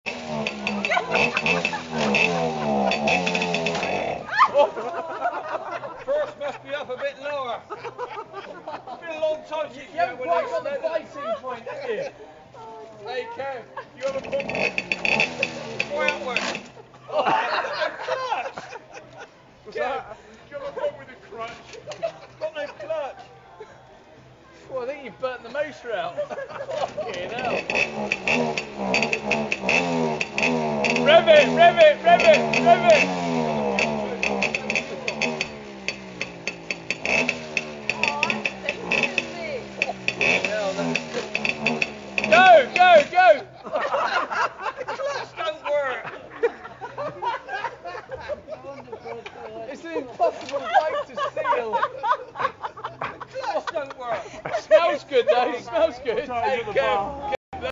Dorset Divots in Portugal (1)
How not to steal a motorbike while on a golfing trip to the Algarve Kev!!